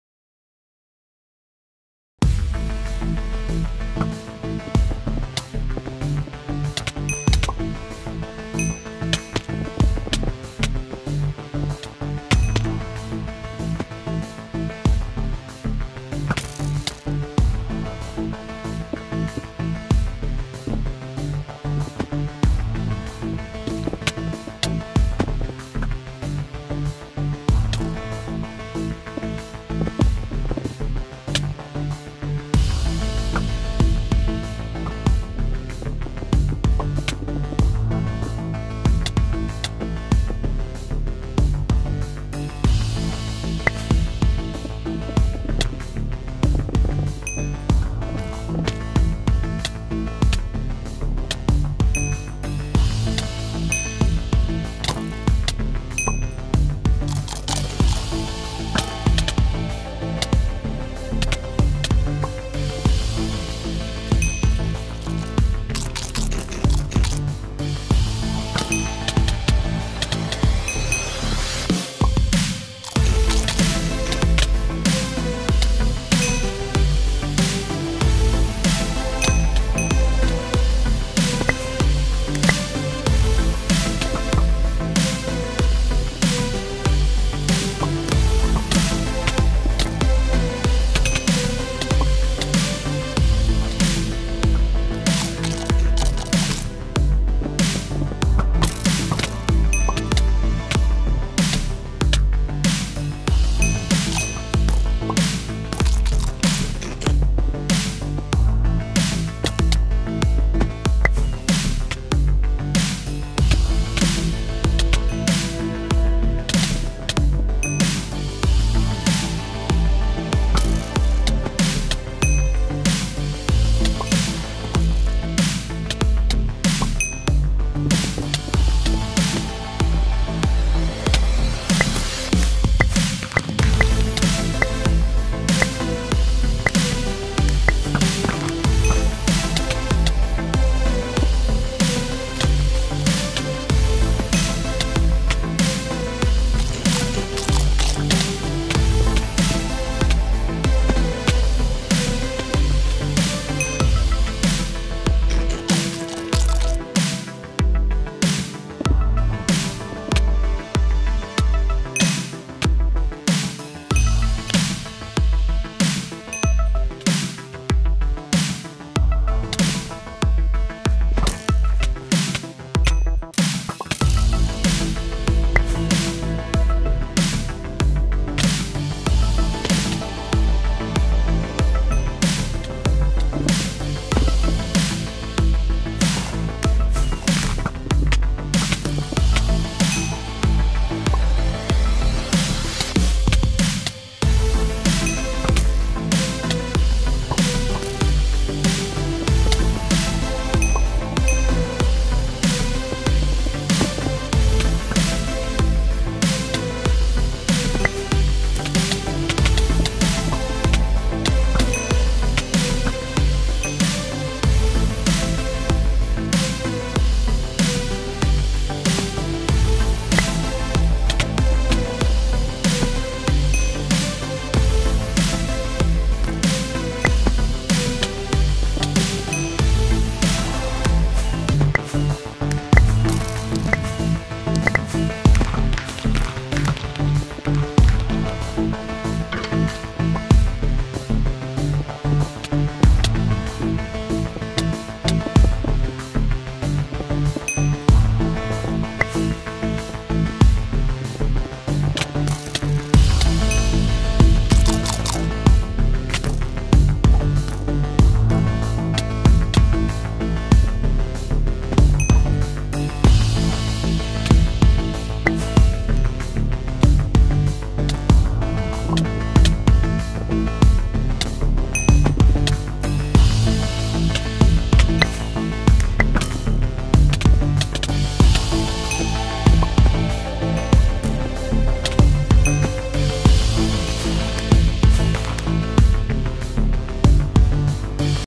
Attempting a Bridge winstreak!
I played with a group of friends. I don't play Bridge often, so this was fun and new!
Also, double upload. Enjoy :)